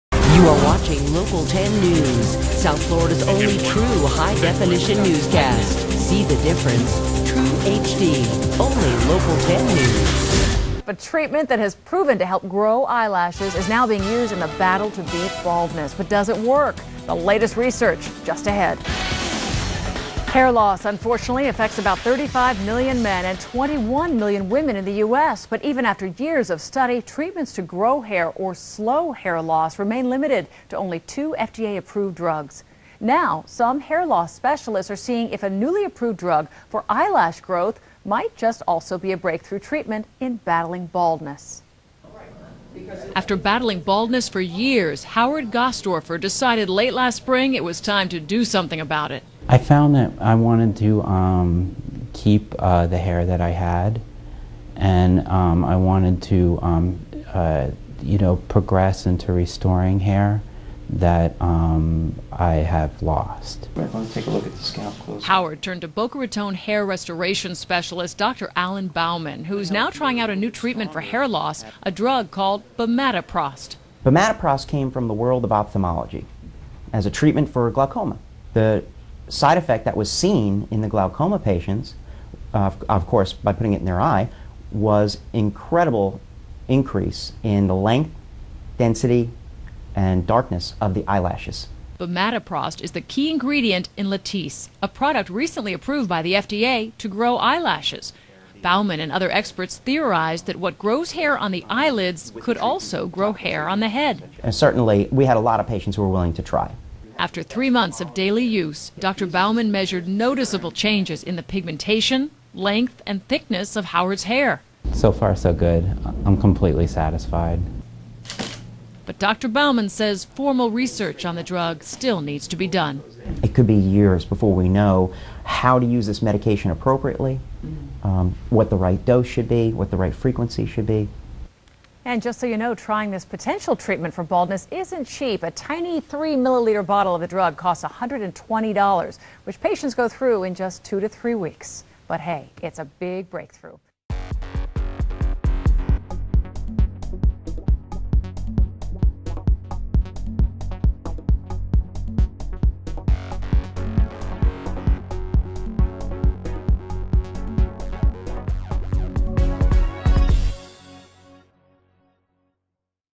Latisse / Bimatoprost for Scalp Hair Loss
-- The prostaglandin-analog Bimatoprost, the active ingredient in Allergan's Latisse, has been FDA-approved to increase eyelash growth.  New research shows that scalp hair follicles contain prostaglandin receptors.  Is Latisse the 'new' rogaine/minoxidil for people with scalp hair loss?  Bauman patient, Howard G., tries the medication and reports some moderate hair growth from the treatment.   Howard is not using any additonal medicaitons (like Propecia/finasteride or Minoxidil) and has not had a hair transplant.  Since this video was posted, Allergan has initiated clinical trials to achieve FDA-approval of bimatoprost for hair growth.